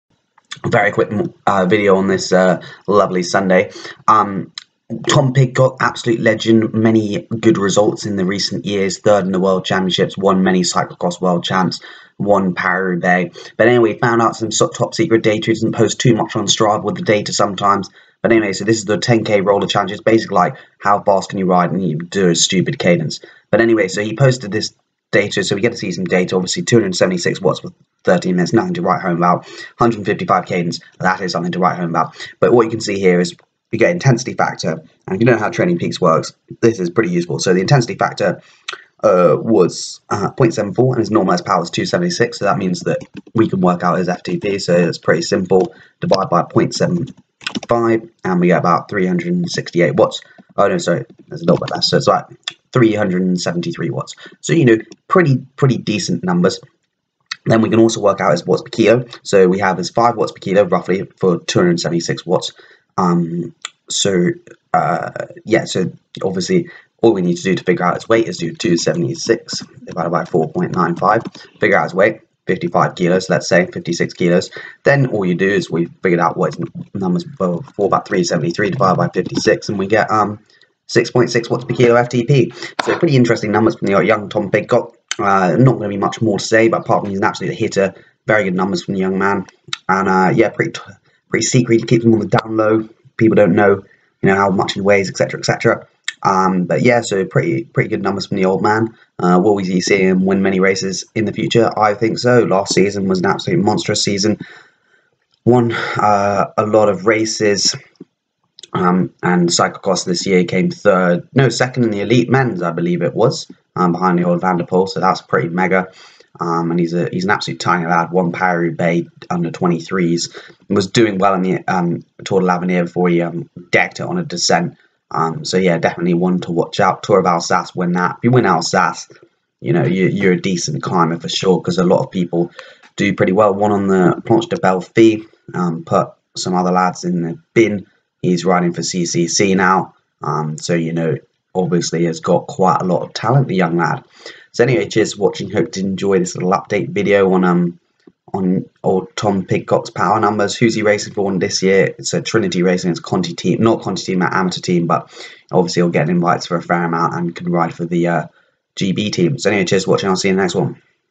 Tom Pidcock's Secret Power Data Revealed!!
Pidock has often kept his power data secret but his is a quick insight into his numbers based on a training peaks snapshot